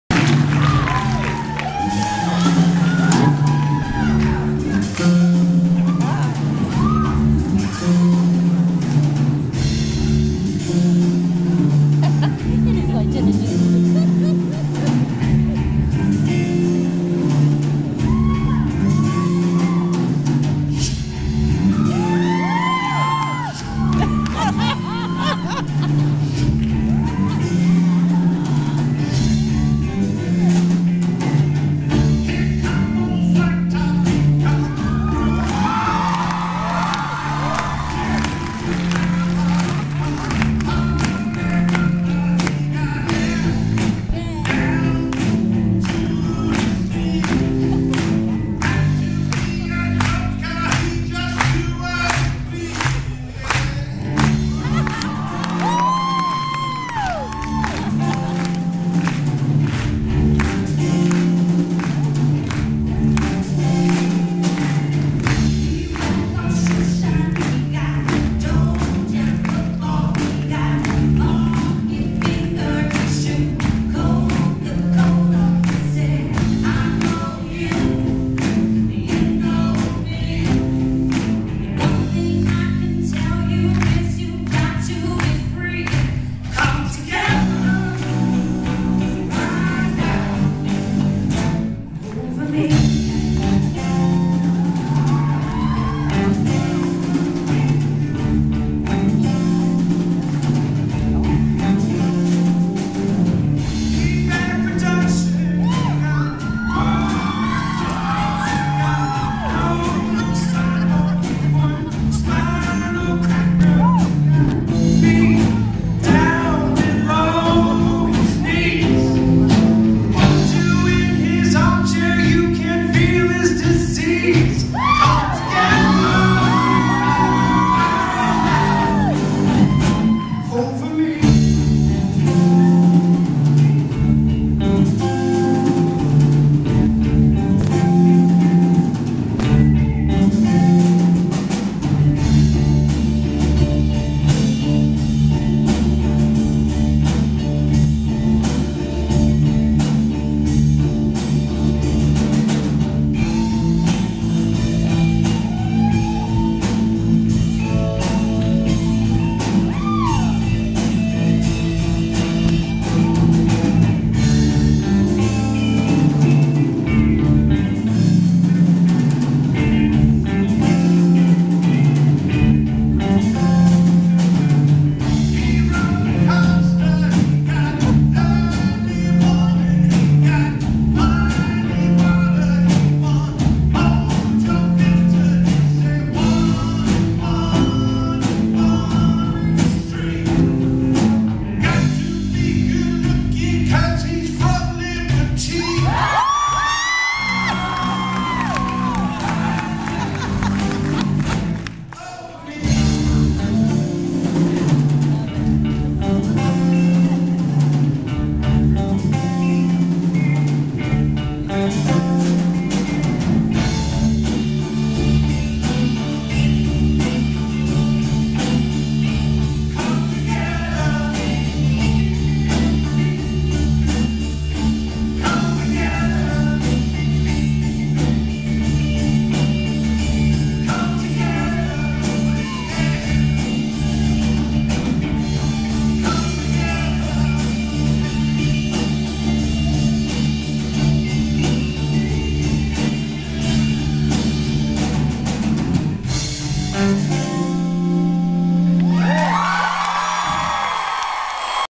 LHS Pops Concert - Educator Band
Principal Walsh and teachers Mrs. Carreon, Mr. Taylor, Mr. Groseclose & Mr. Carreon